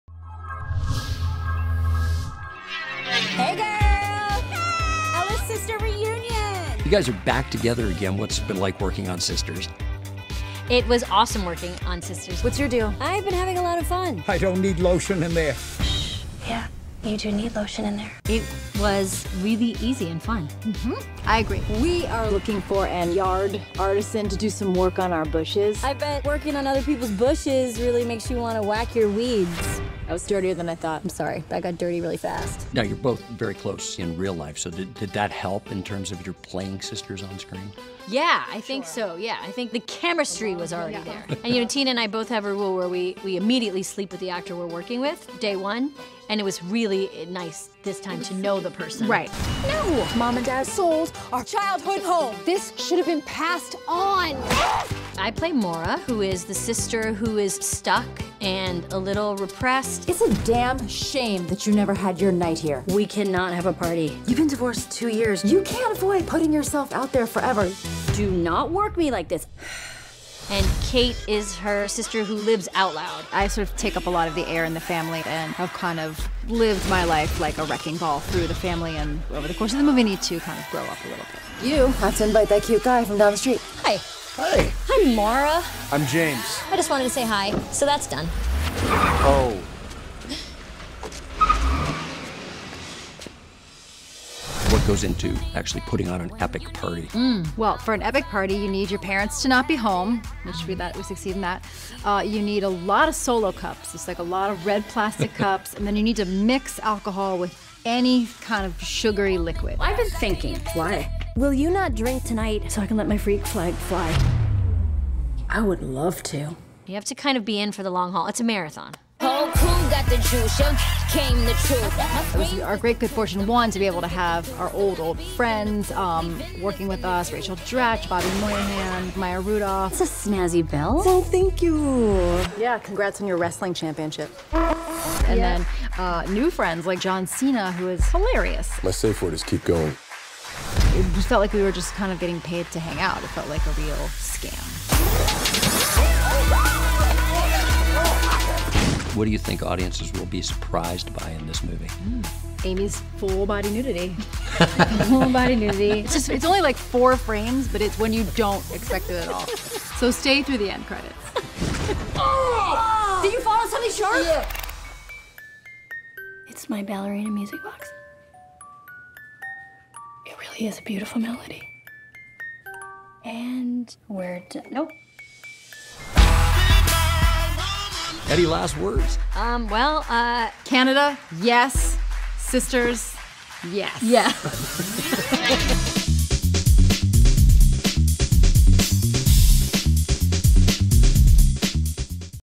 Amy Poehler and Tina Fey on Sisters
We sat down with BFFs Amy Poehler and Tina Fey to talk about their latest film, Sisters.

Two sisters decide to throw one last house party before their parents sell their family home. (IMDB)

SISTERS stars Tina Fey, Amy Poehler, Kate McKinnon and Ike Barinholtz.